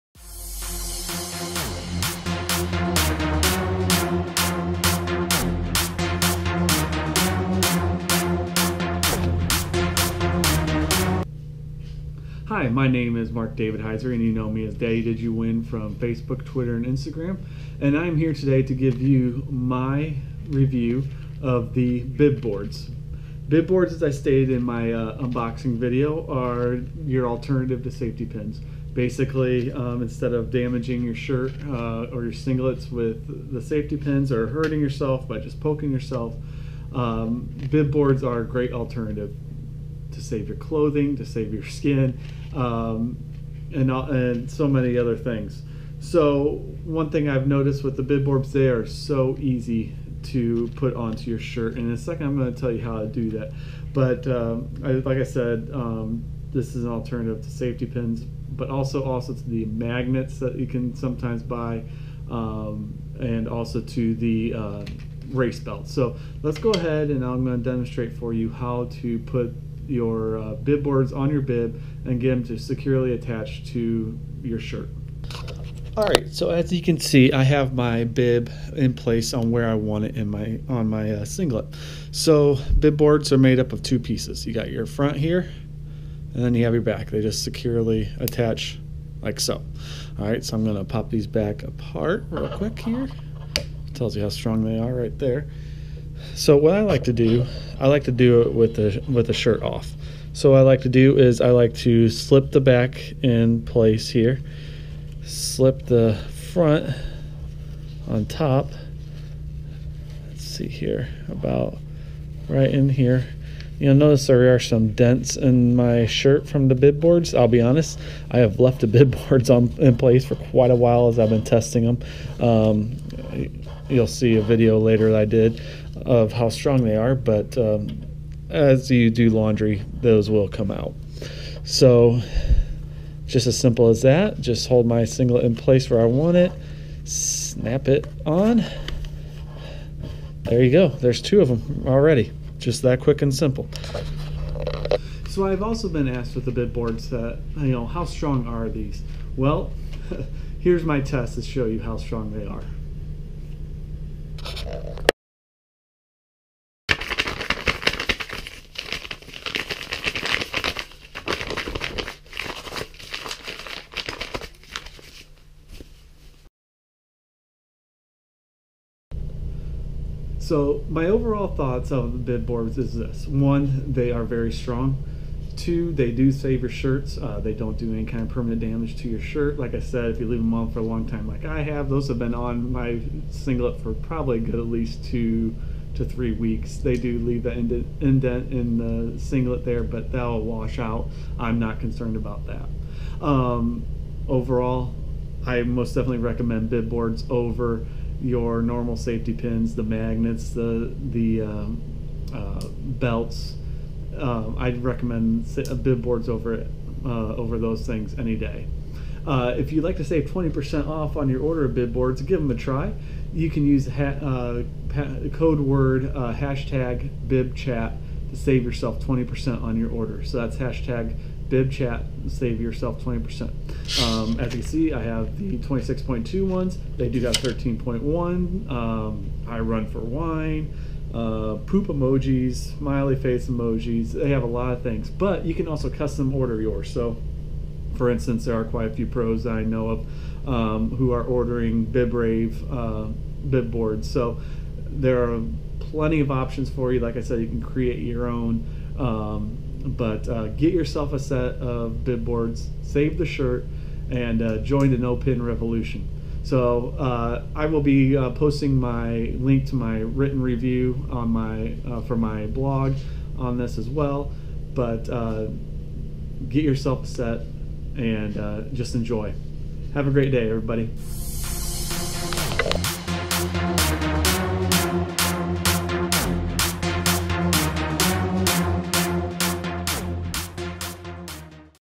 Product Review: BibBoards
Tired of your running clothes being ruined by using safety pins for your race bibs?  Give BibBoards a try.  Easy to use and no damage to your clothes!